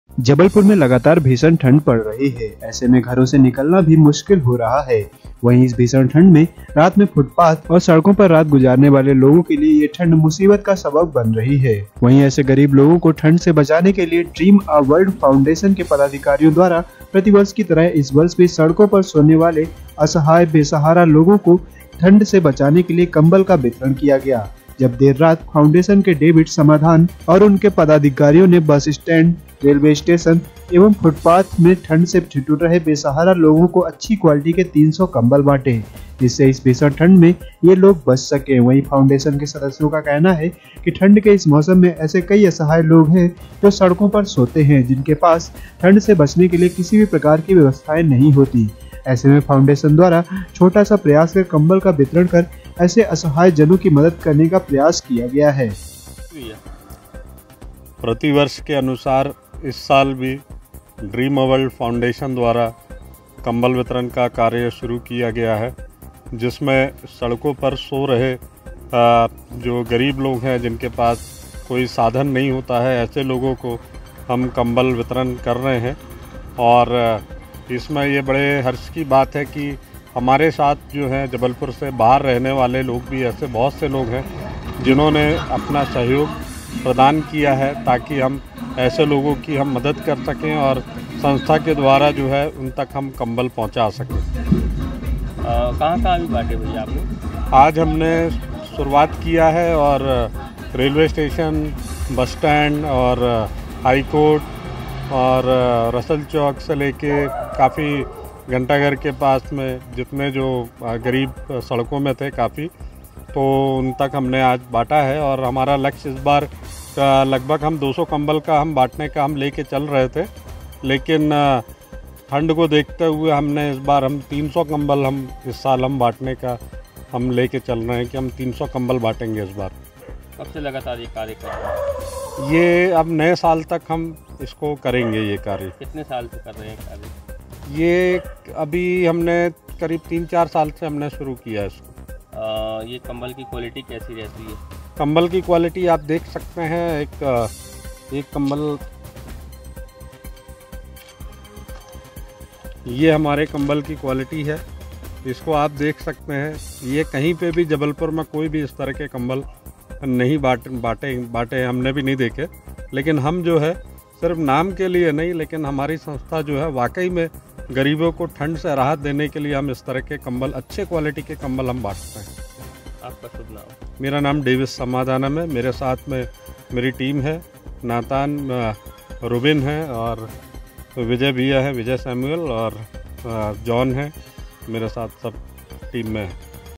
भीषण ठंड को देखते हुए ड्रीम आ फाउंडेशन ने मानवता की मिसाल पेश, बांटे कंबल
जबलपुर में लगातार भीषण ठंड पड़ रही है ऐसे में घरों से निकलना भी मुश्किल हो रहा है वही इस भीषण ठंड में रात में फुटपाथ और सडको पर रात गुजारने वाले लोगो के लिए ये ठंड मुसबीत का सबब बन रही है,
वही ऐसे गरीब लोगों को ठंड से बचाने के लिए ड्रीम आ वर्ल्ड फाउंडेशन के पदाधिकारियों के द्वारा प्रतिवर्ष की तरह इस वर्ष भी सडको पर सोने वाले असहाय,बेसहारा लोगो को ठंड से बचाने के लिए कंबल का वितरण किया गया, जहा देर रात फाउंडेशन के डेविड समाधान और उनके पदाधिकारियों ने बस स्टैंड,स्टेशन,एवं फुटपाथ में ठंड में ठिठुर रहें बेसहारा लोगो को अच्छी क्वालिटी के 300 कंबल बांटे, जिससे इस भीषण ठंड से ये लोग बच सके।
वही फाउंडेशन के सदस्यों का कहना है की ठंड के इस मौसम में ऐसे कई असहाय लोग है जो सडको पर सोते है जिनके पास ठंड से बचने के लिए किसी प्रकार की व्यवस्थाएं नही होती ऐसे में फाउंडेशन के द्वारा छोटा सा प्रयास कर कंबल का वितरण कर ऐसे असहाय जनों की मदद करने का प्रयास किया गया है।